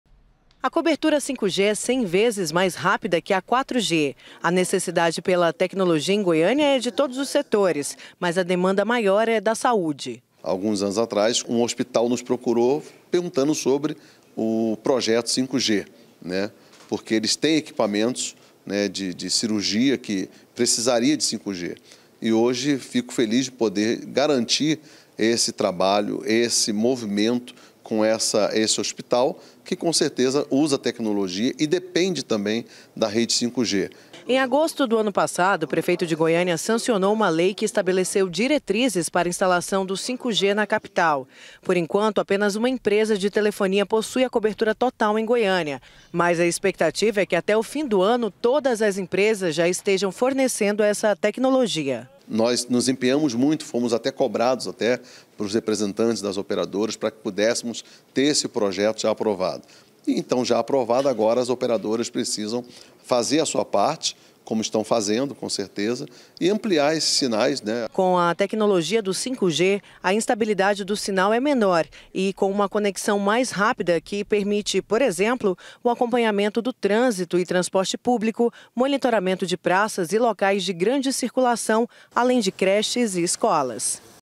INTERNET 5G: OPERADORAS VÃO AMPLIAR COBERTURA EM TODA A CAPITAL
ATÉ O FIM DO ANO, TODAS AS OPERADORAS DE CELULAR VÃO OFERECER COBERTURA DE INTERNET 5G EM GOIÂNIA. HOJE, APENAS UMA EMPRESA TEM SINAL EM 100% DA CAPITAL.